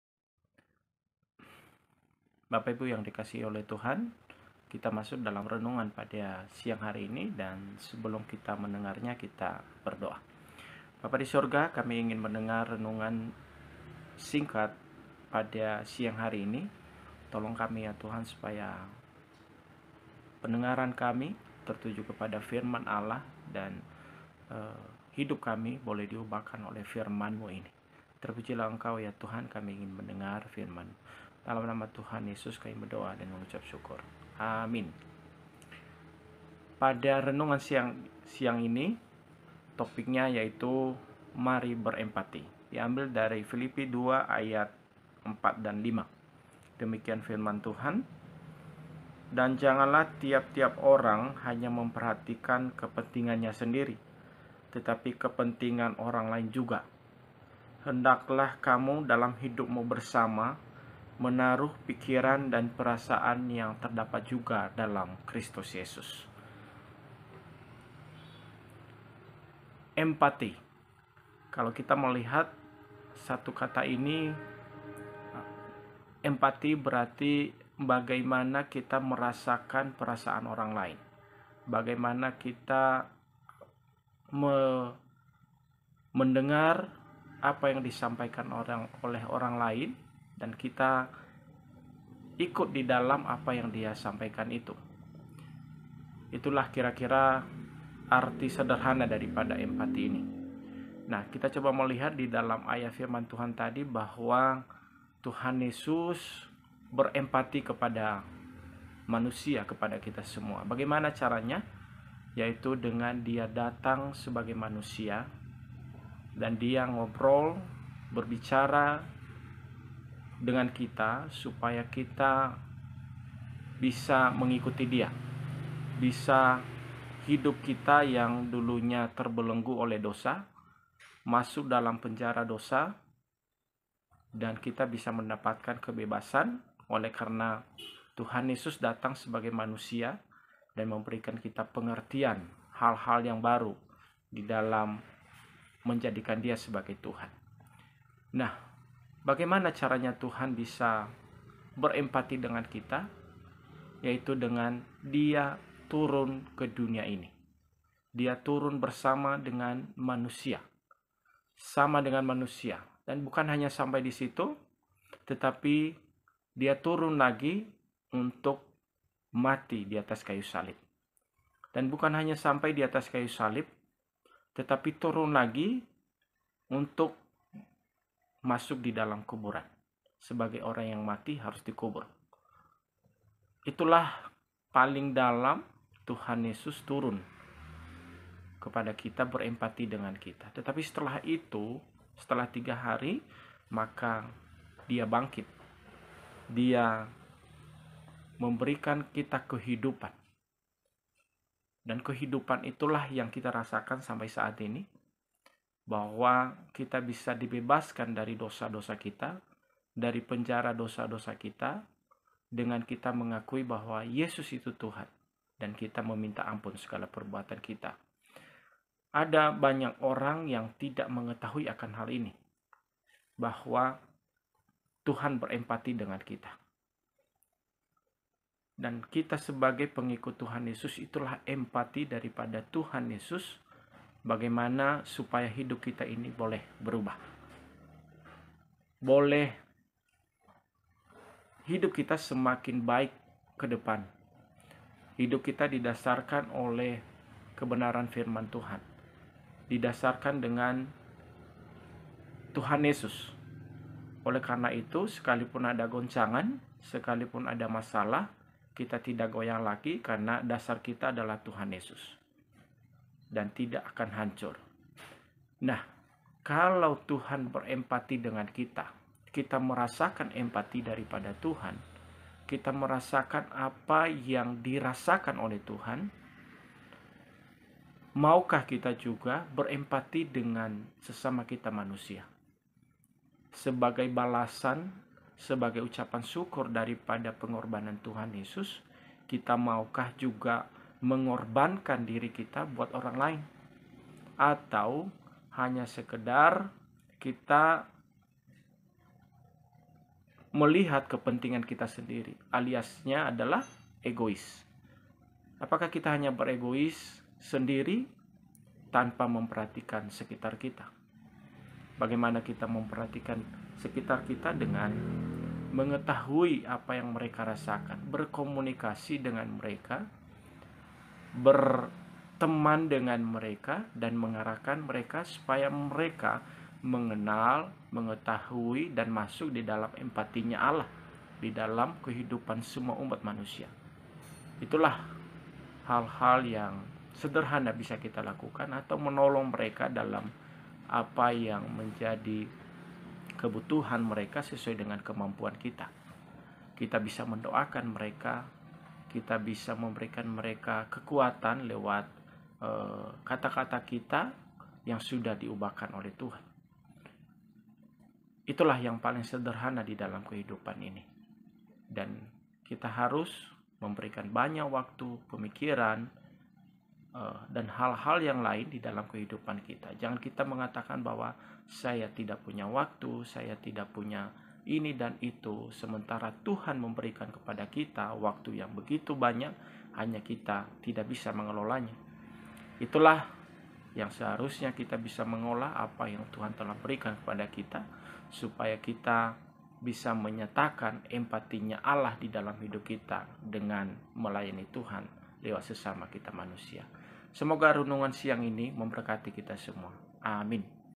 Mari Berempati | Bpk. Idaman Hati Gea
Saudara-saudara yang terkasih, banyak orang yang tidak tahu bahwa Tuhan Yesus telah berempati dengan kita. Sebagai pengikut Kristus, maukah kita juga berempati kepada sesama kita dan berkorban bagi orang lain? Pada renungan singkat hari ini, kita mau sama-sama belajar bagaimana kita dapat berempati kepada sesama kita. Tuhan Yesus memberkati.